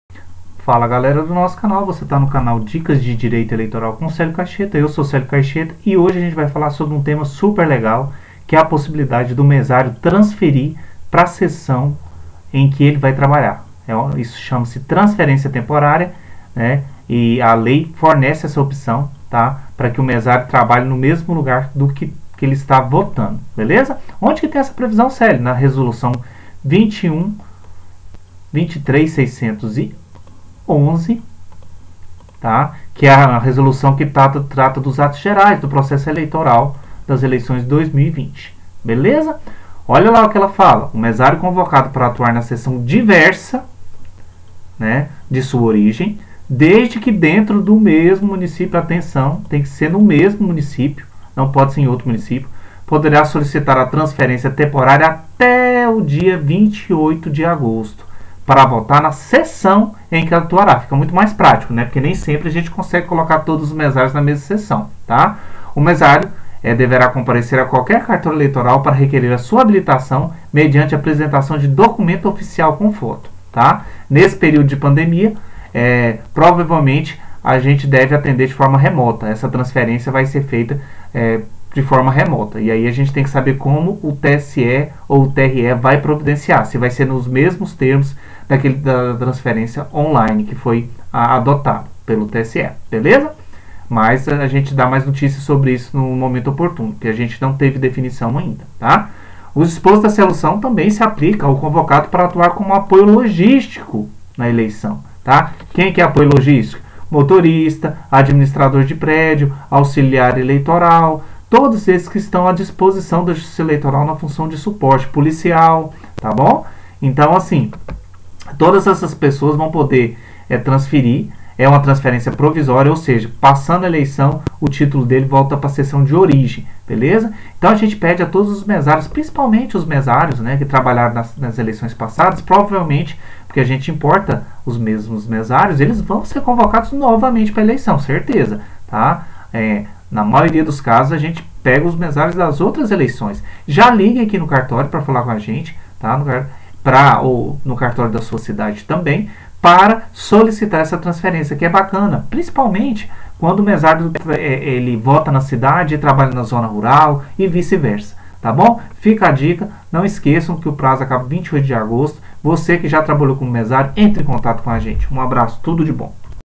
Mesário pode solicitar a transferência para seção em que trabalhará até o dia 28 de agosto de 2020.
Canal tem o intuito de trazer informações aos partidos políticos e eleitores bem como boas práticas de procedimentos cartorários aos servidores da Justiça Eleitoral.